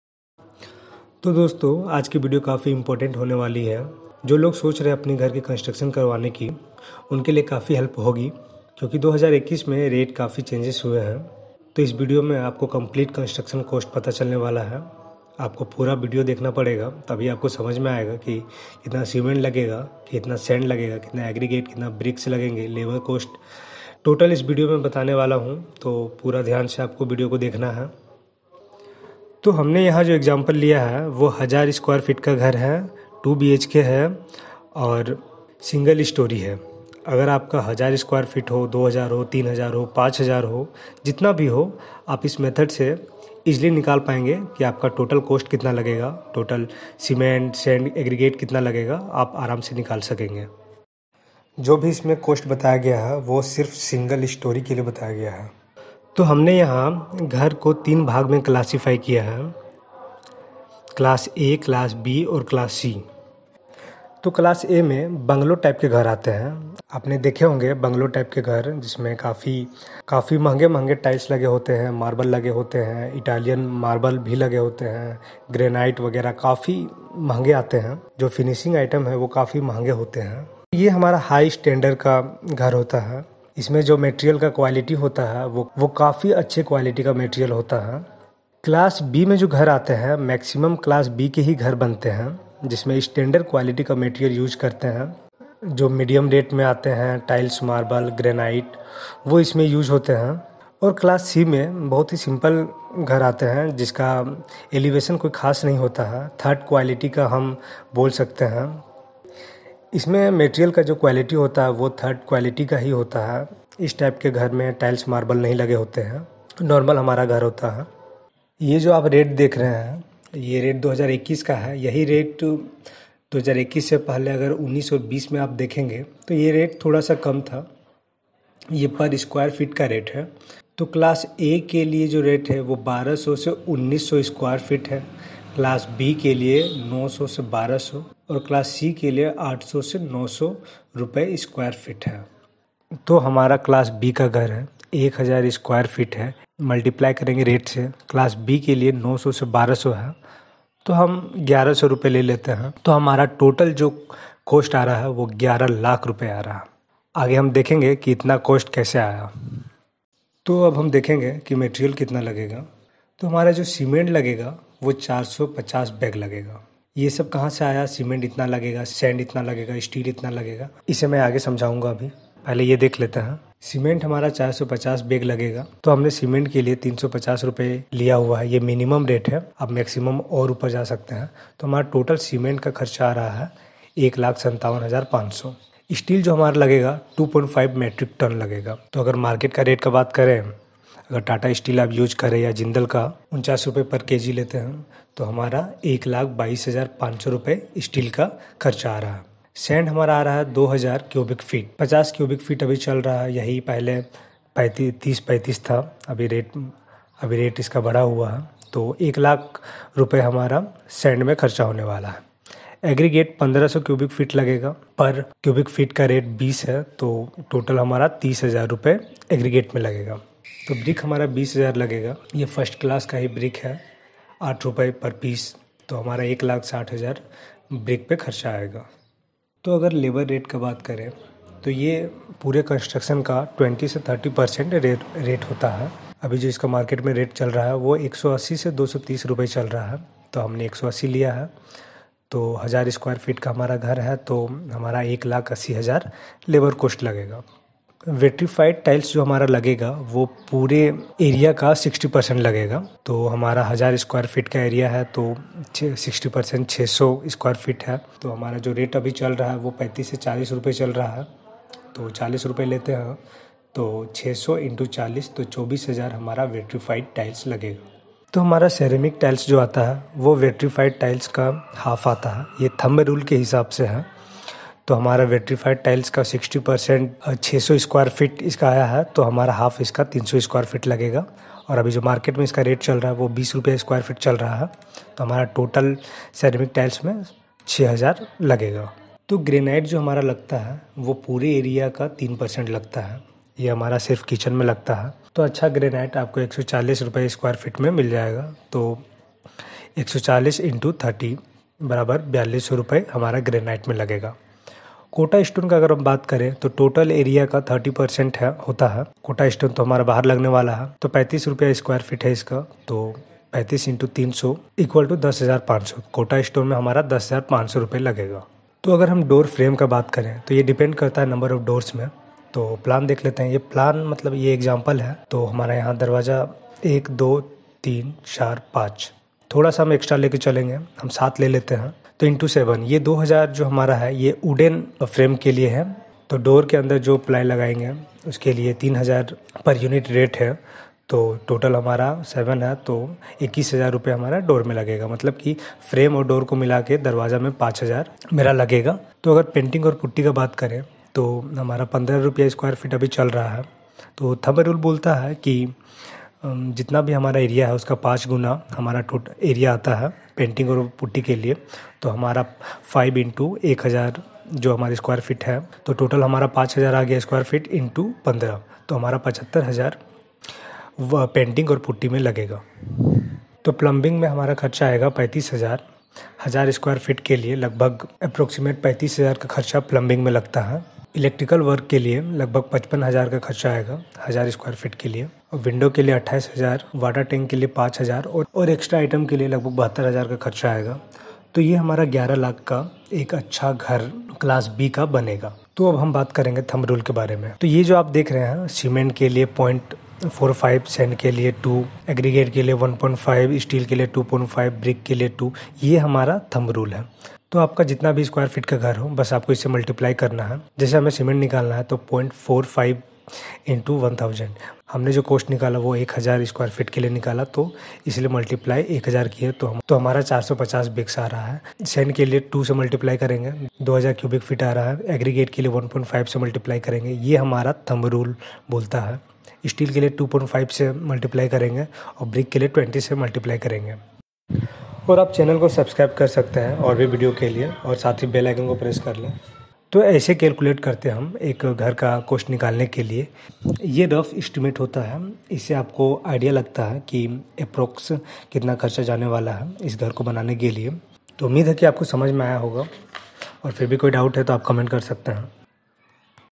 Construction cost of 1000 square feet house in 2021 | Rate analysis for house construction
you can see detail about rate analysis of house construction .
Complete construction cost of 1000 sqft ..

you can also see about quantity of building material required for 1000 sqft
thumb rule for different building material percentage